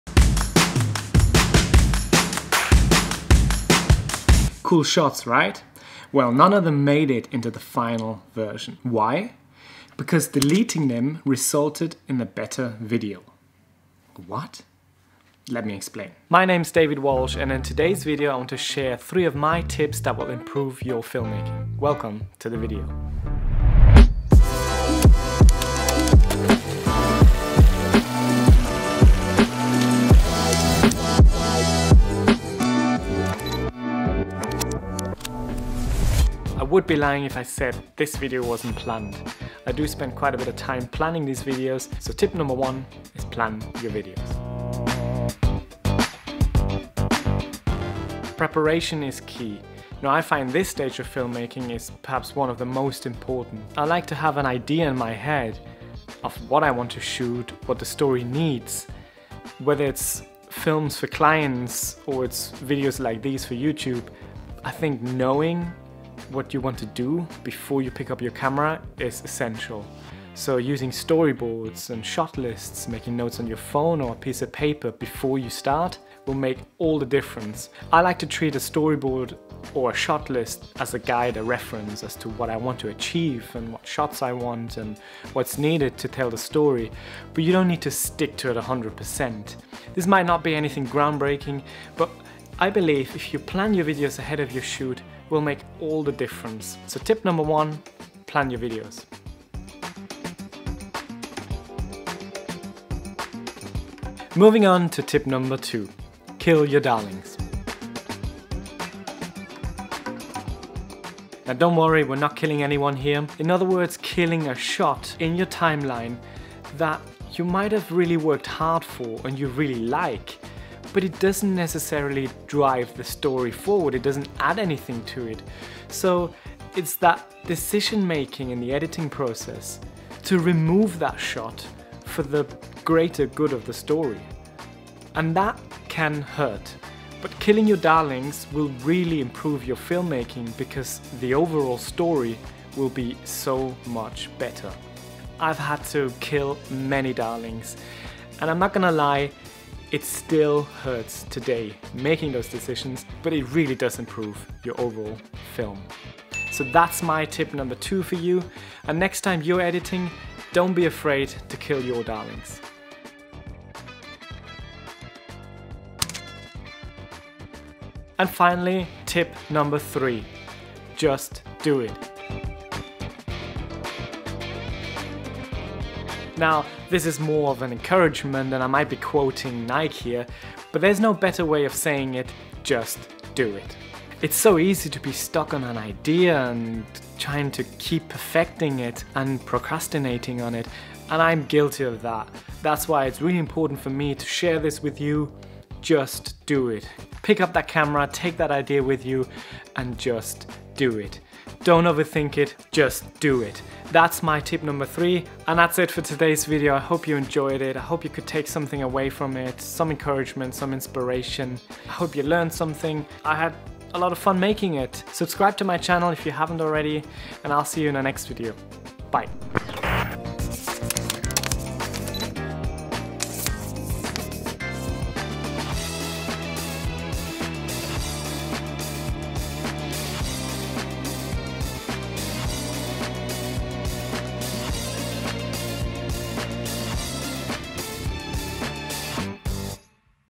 Make BETTER videos! 3 pro tips
3 tips to improve your videos immediately! Make your videos stand out! Simple tricks and tips that will take your videos to the next level. How you can maximize views on your videos.

Learn how to make better videos.